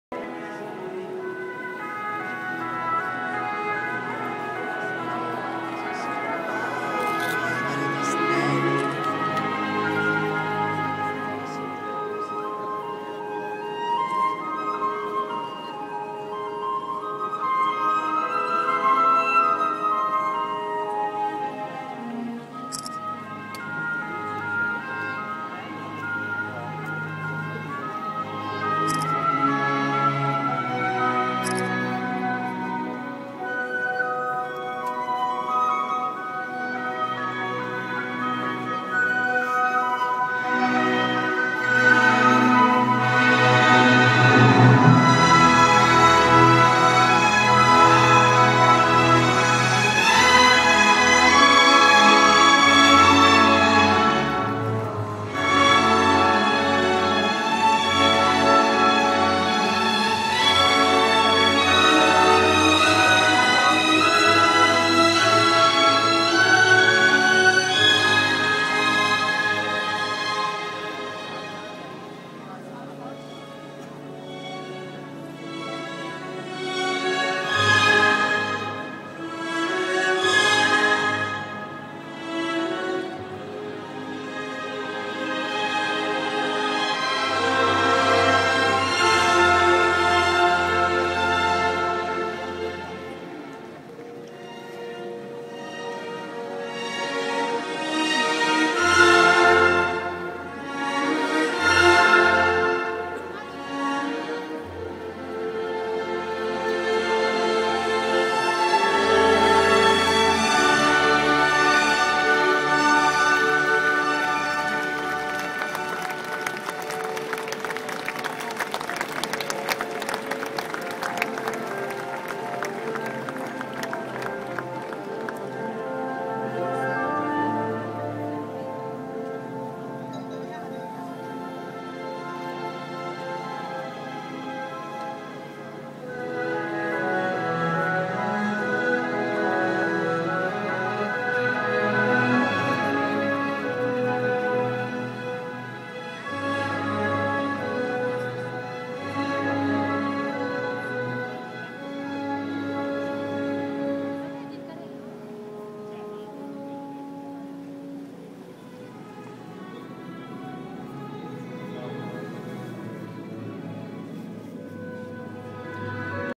LAC Lugano Arte e Cultura opening ceremony
Opening ceremony of LAC in Lugano, Ticino, Switzerland on September 12th 2015